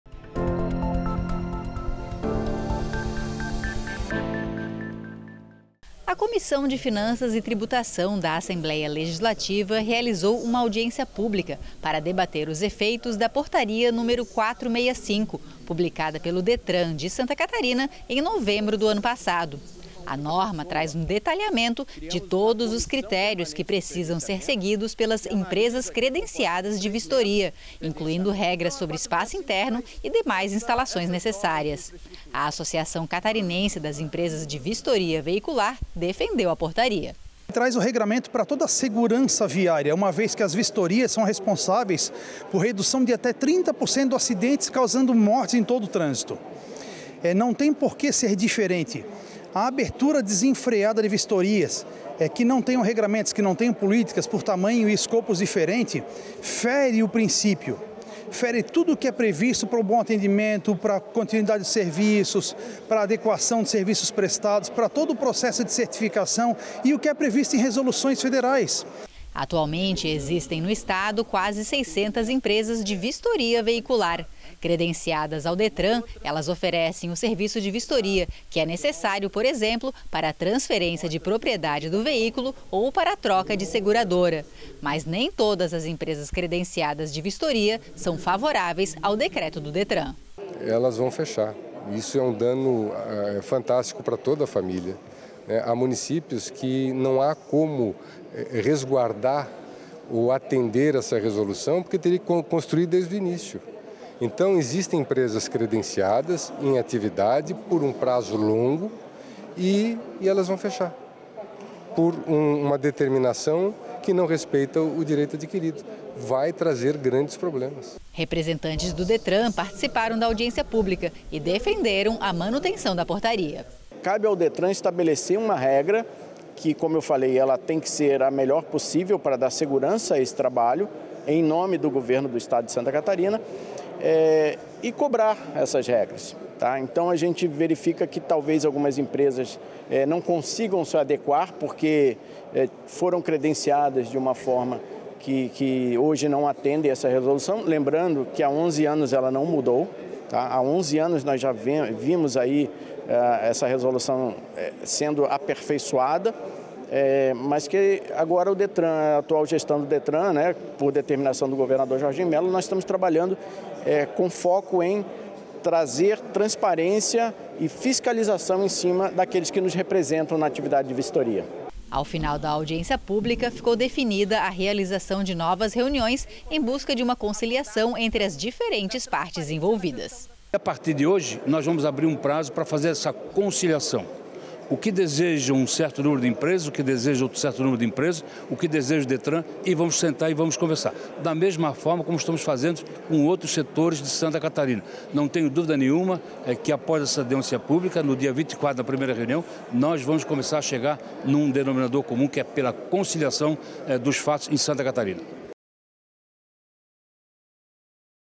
Vistoria veicular é tema de audiência pública no parlamento - 17/06/24
A Comissão de Finanças e Tributação da Assembleia Legislativa realizou uma audiência pública para debater os efeitos da Portaria nº 465, publicada pelo Detran de Santa Catarina em novembro do ano passado.  A norma traz um detalhamento de todos os critérios que precisam ser seguidos pelas empresas credenciadas de vistoria, incluindo regras sobre espaço interno e demais instalações necessárias.